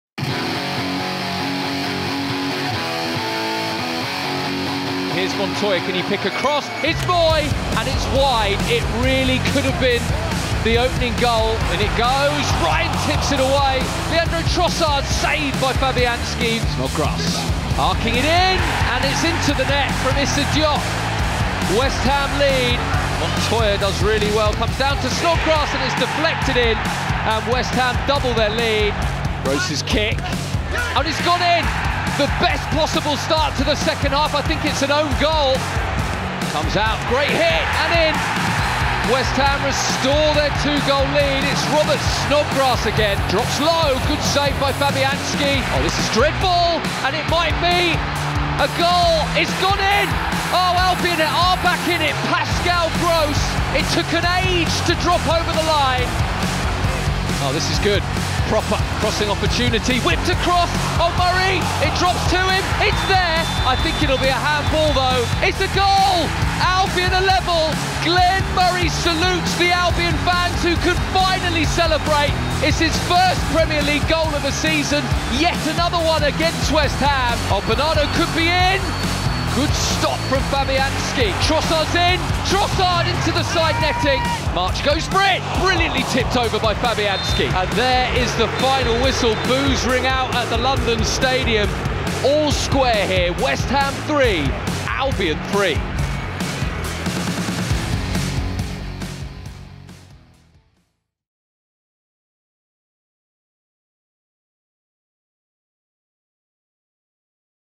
West Ham Montage
Glenn Murray sealed a memorable comeback in a thriller at the London Stadium.